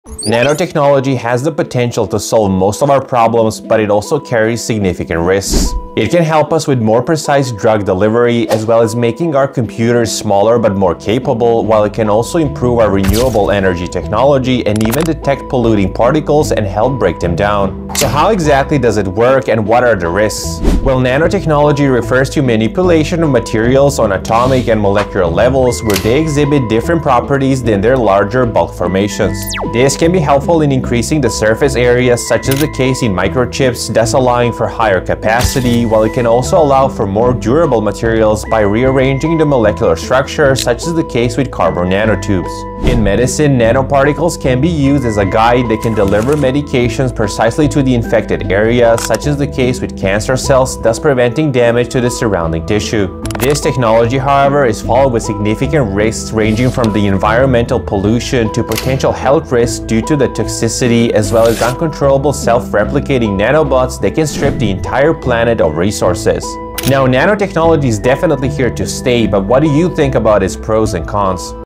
Will This Technology Save Or Destroy Humanity!?🤔😱shortvideo
Nanotechnology is showing increasing potential to tackle some of humanity's biggest challenges including diseases and pollution but it also carries significant risks.